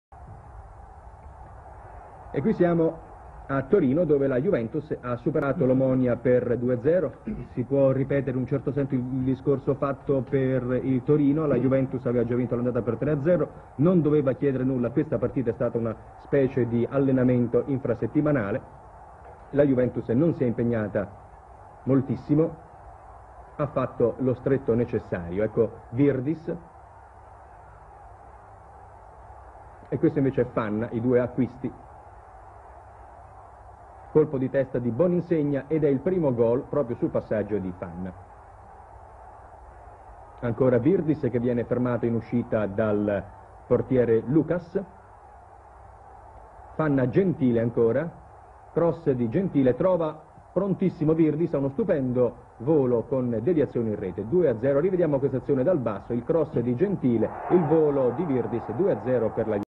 ECC 1977-78. 1 Round. Juventus - Omonia Nikosia. Highlights.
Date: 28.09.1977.
Local time: 20:30.
Stadium: Comunale (Torino / Italy).
Attendance: 18000.
Referee: Marcel Hermann (Luxembourg).
Juventus: Giancarlo Alessandrelli, Luciano Spinosi, Gaetano Scirea, Claudio Gentile, Antonio Cabrini, Marco Tardelli, Giuseppe Furino (Vinicio Verza 46'), Romeo Benetti, Pietro Virdis, Pietro Fanna, Roberto Boninsegna; manager Giovanni Trapattoni.
Omonia Nikosia: Loukas Andreou (Dimos Eleftheriadis (yel. card 88') 46'), Koulis Poiatzis, Nicos Patikkis, Antonas Paraskeva, Takis Miamiliotis, Klitos Erotokritou, Gregory Savva, Takis Mavris (Sotiris Tsikkos 23'), Nikos Charalambous, Andreas Kanaris, Sotiris Kaiafas.
Goals:
1 : 0 Roberto Boninsegna 12.
2 : 0 Pietro Virdis 75.